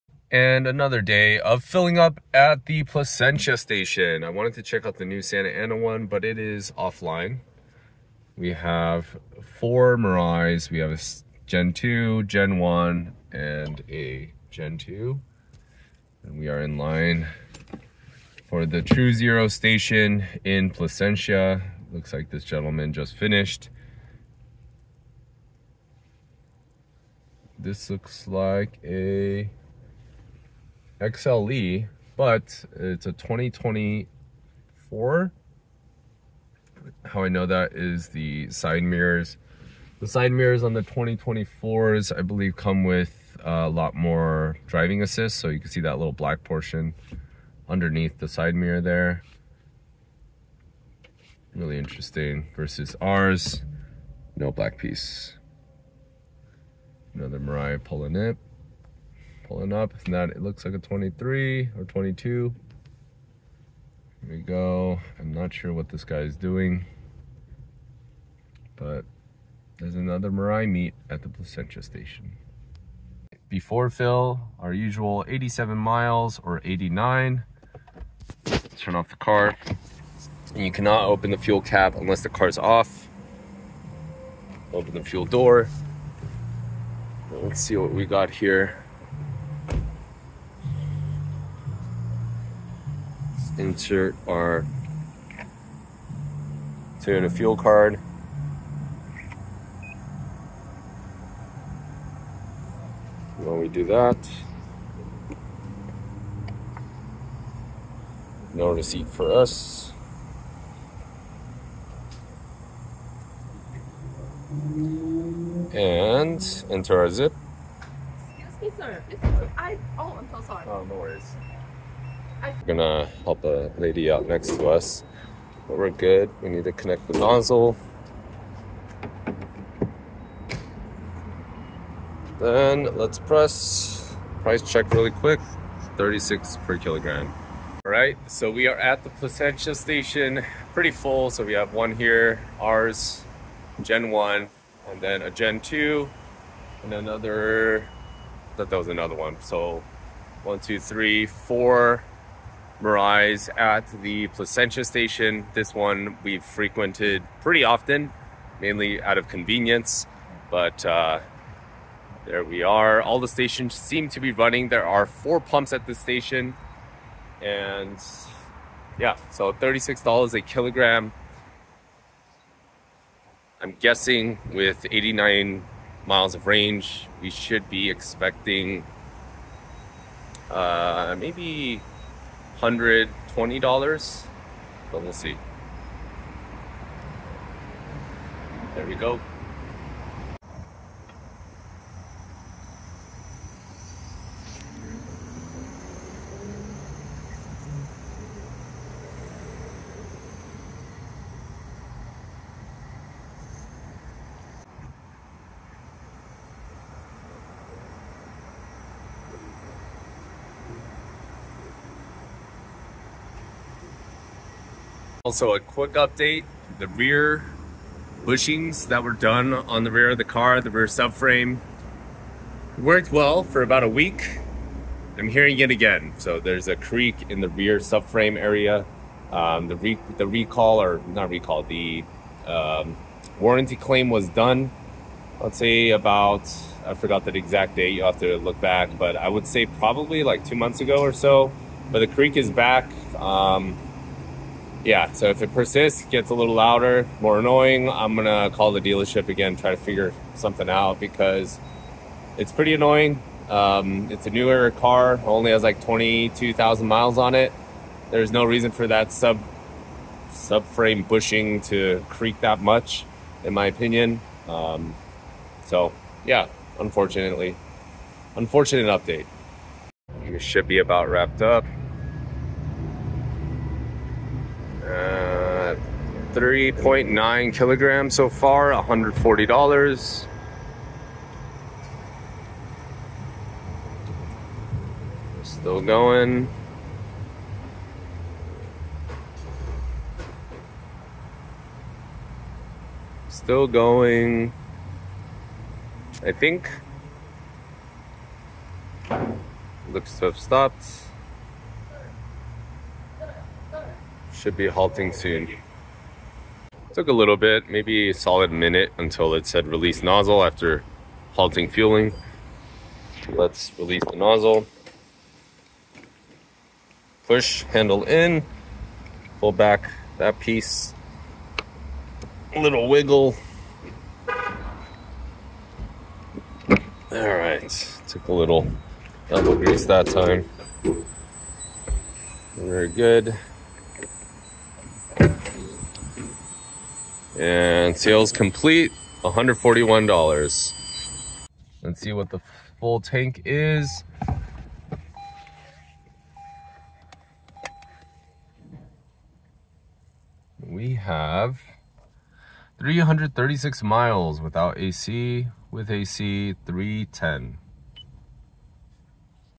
another day filling up a hydrogen car
Filling up smoothly at the True Zero station in Placentia, CA